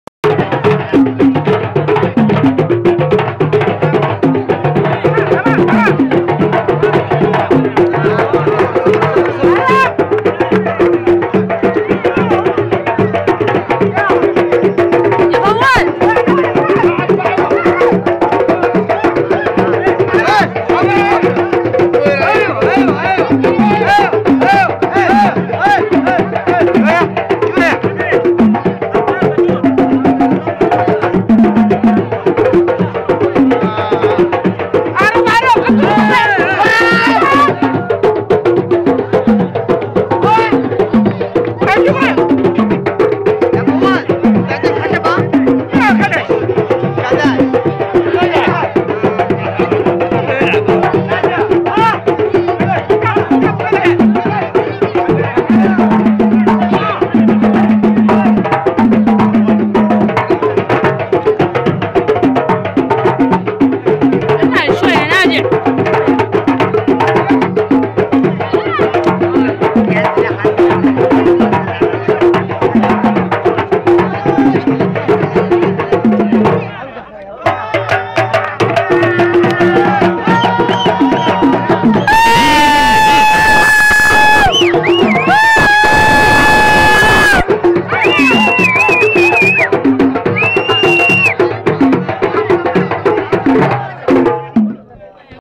زواج عادل جبران3 قريه الخرادله.mp4
لعب حامي في زواج عادل جبران قريه الخرادله
تم الرفع بواسطه عبدالله زيلعي منتديات قريه الخرادله